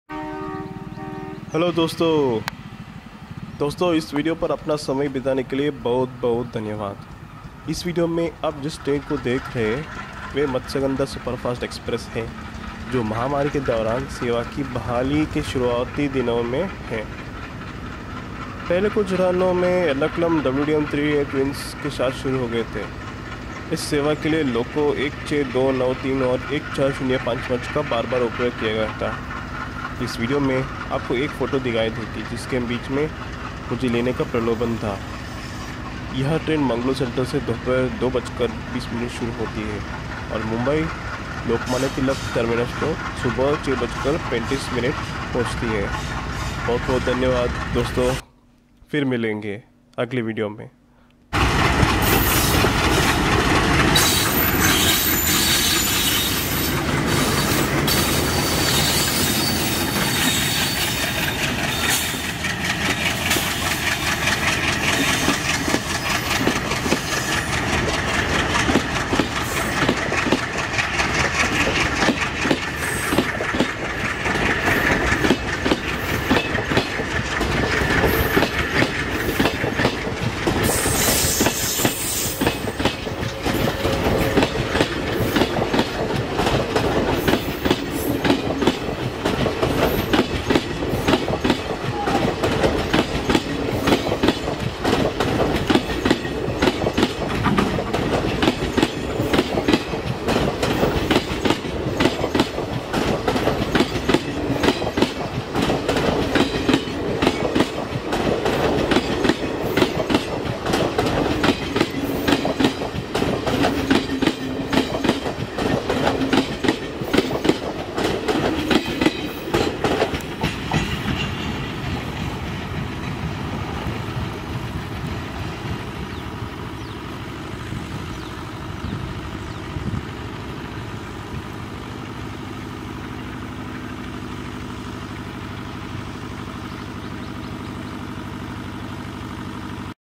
Classical ERS Twin ALCO departure with Iconic Matsyagandha Superfast Express at Udupi-Konkan Railway
It was those days, which had brought the reminiscence of its initial runs, originally running with Ernakulam WDM2 twins, The days of melodious chugging accompanied with pure nostalgic vibes! Never felt that the journey should end, because it was something which gave us absolute peace in mind! Back in Late 2020, The service was restored after it took a lot of rest during the pandemic! In the first week of runs, Every Alternate day had the very same pair as seen in the video! On the other days, Its present link, WDP4D took charge! But the twin ALCOs ran only for maybe 4-5 trips and then Matysagandha had its link regularised.

16293 + 14055  ERNAKULAM WDM3A TWINS - 02620 MATSYAGANDHA SUPERFAST EXPRESS!

About Konkan Railway Electrification update in Udupi; Electrification work has completed at this place and there have been Trial runs of Electric Locomotives 5 times. The line is expected to be commissioned by May 2021!
One with AJJ WAP-4 and Another run with RPM WAP-7 (CRS RUN) at MPS!

Konkan railway electrification work, Konkan railway electrification trial,
00:00 Hi
00:03 Thanks for clicking!